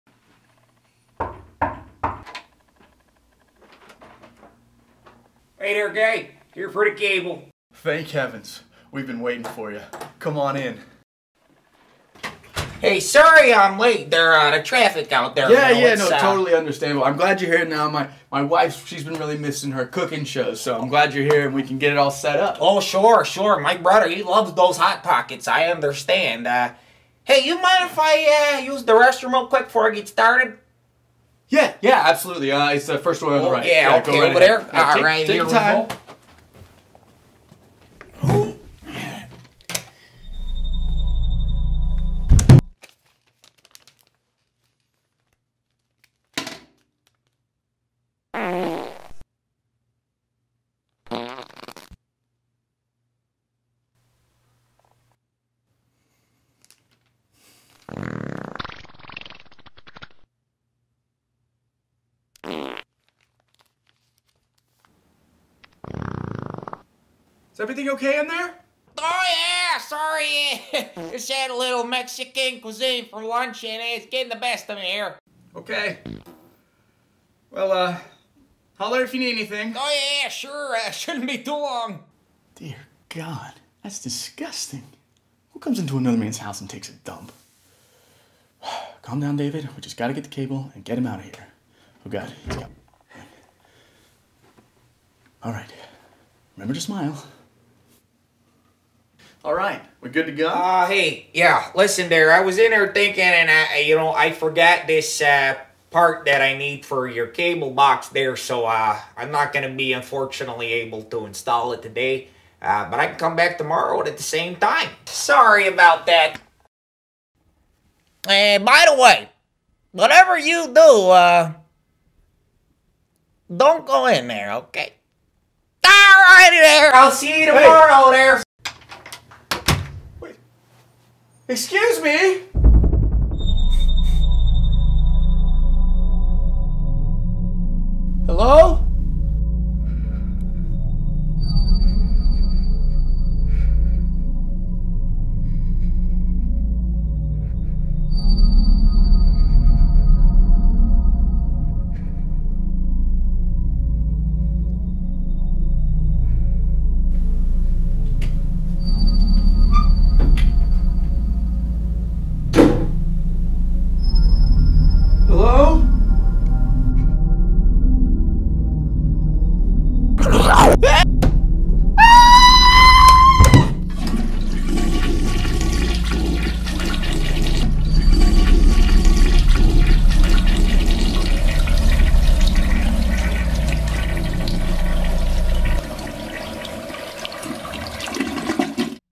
SKIT: Paranormal Poop | James Boyd
Ever have a cable guy come in and use your restroom?...How about using it for number 2? It's totally understandable don't get me wrong everyone has to go sometime, but if you use someones toilet, wash your hands, turn on the fan...and don't leave anything behind...please.